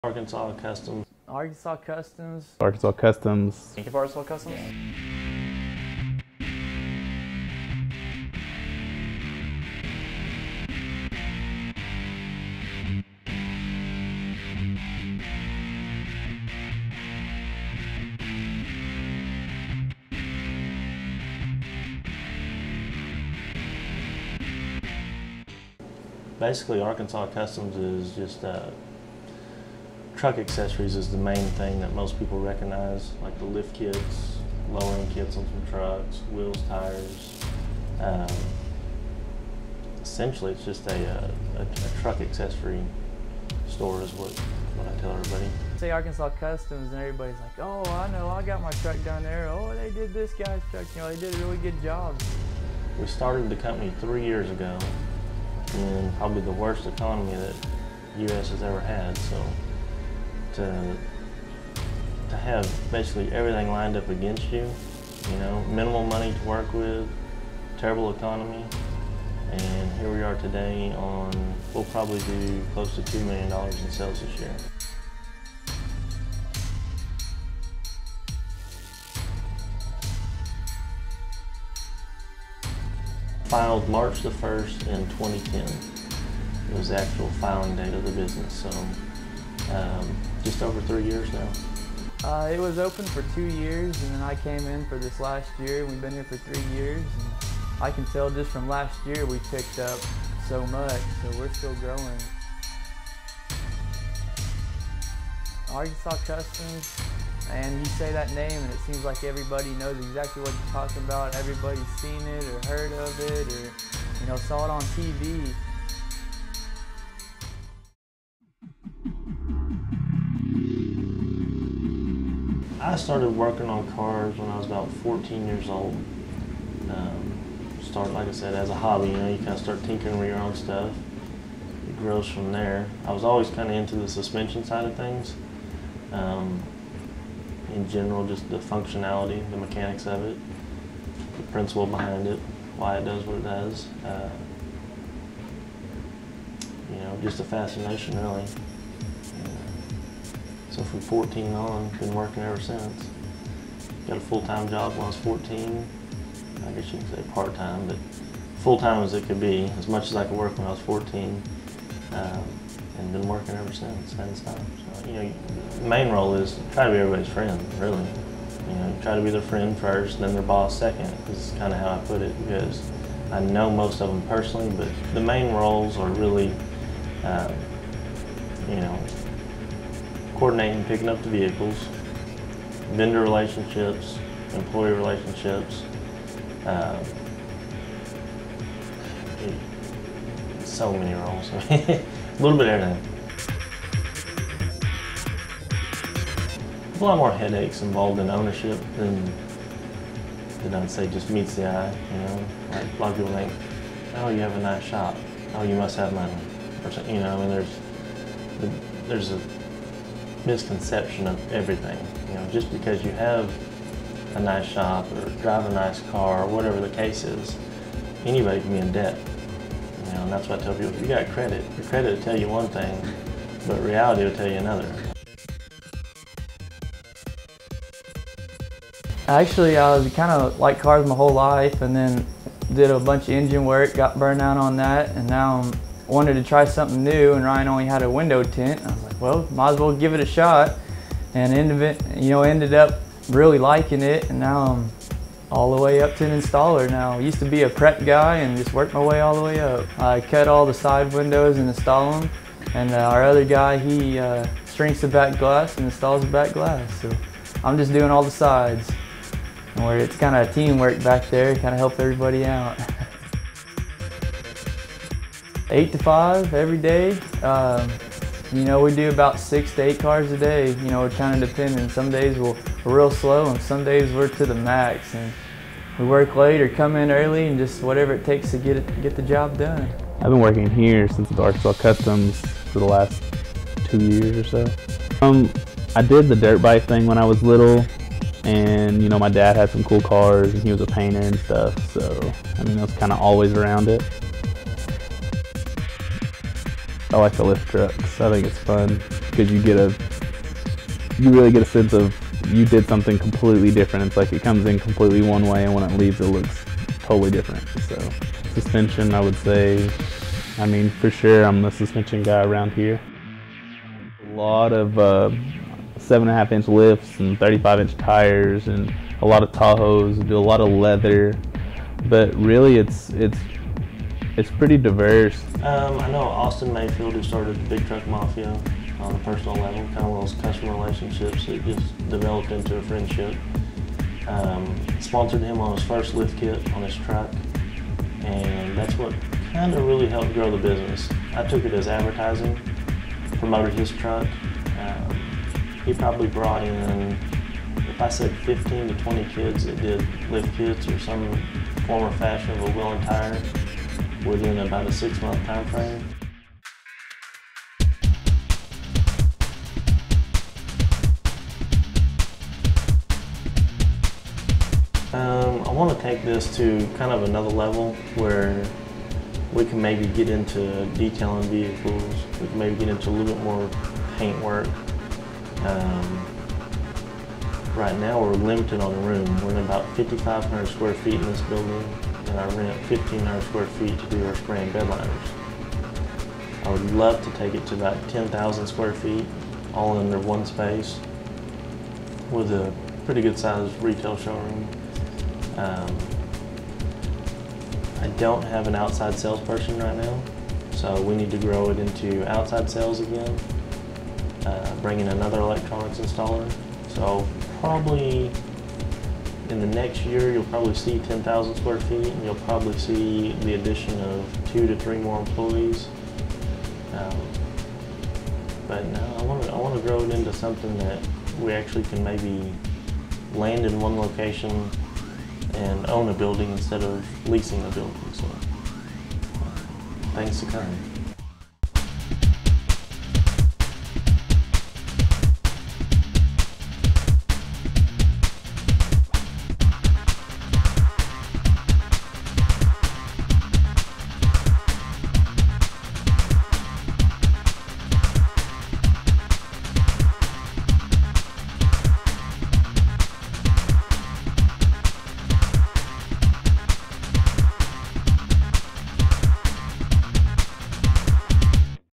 Arkansas Customs Documentary
A documentary made for Arkansas Customs, made by Har-Ber High School WETV.